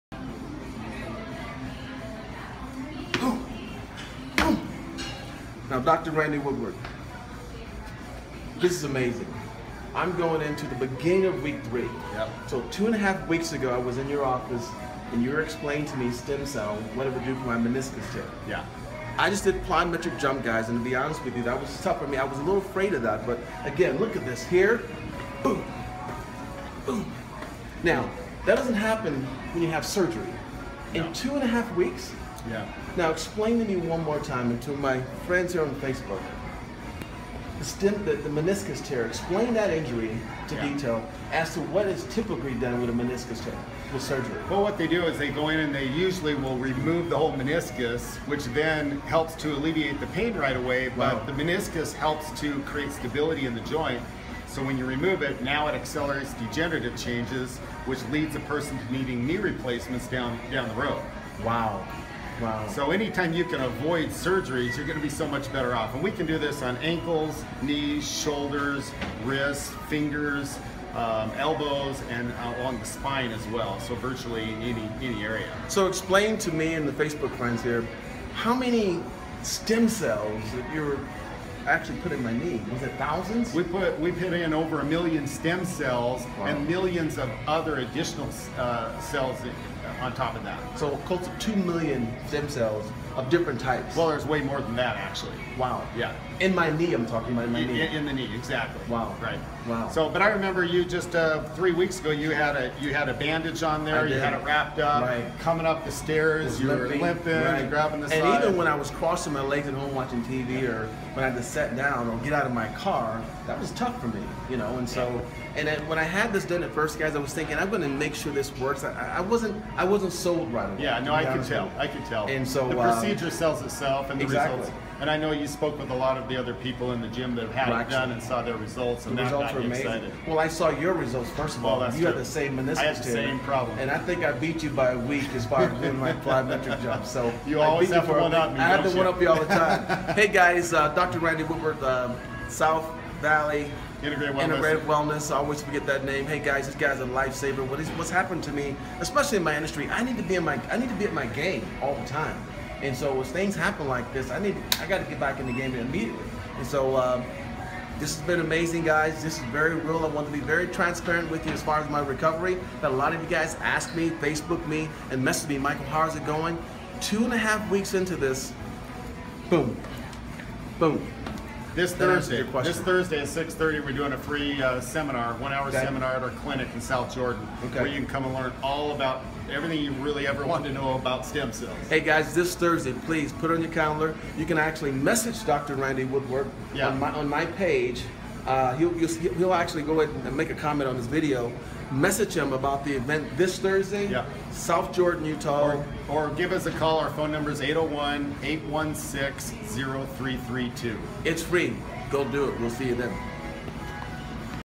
Stem cell meniscus tear 2 weeks post injection-Michael Banks
Michael was considering knee surgery to repair a torn meniscus. He couldn't walk up or down stairs without limping or experiencing severe pain. Watch his improvement after only 2 weeks post stem cell injection!